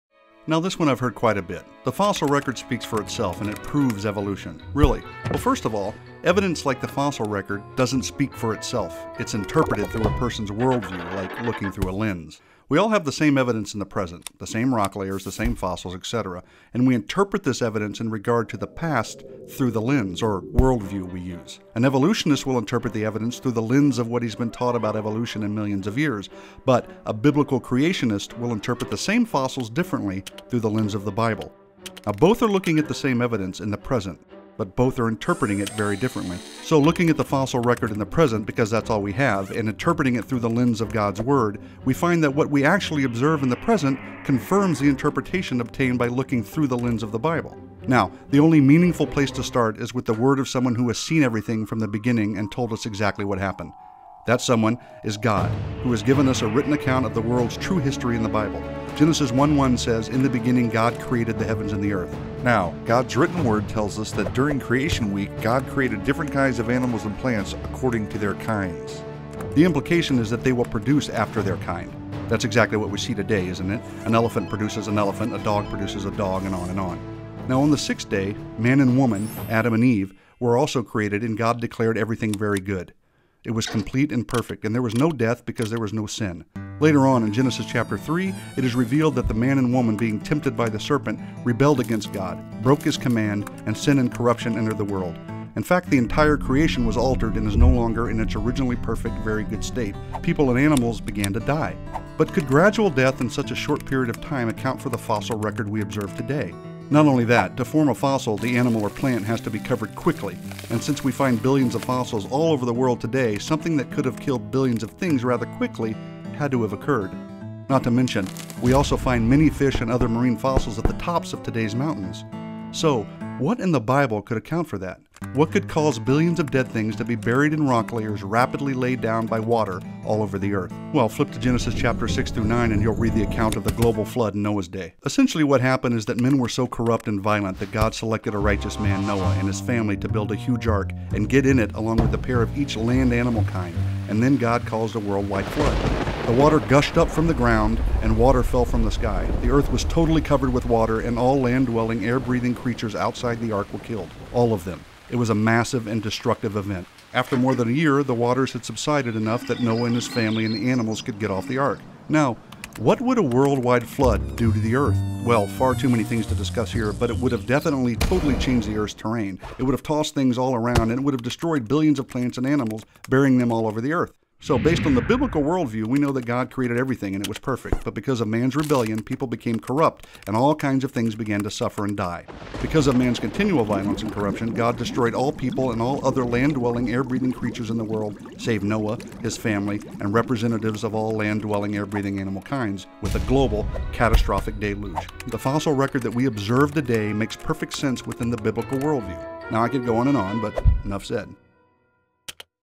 Check This Out: Fossils & the Flood
Is the fossil record "evidence" of evolution, or does a worldwide flood best explain it?
Does evidence "speak for itself" or is it interpreted by people with different worldviews?  Watch this video, and find out a few things you might not have known!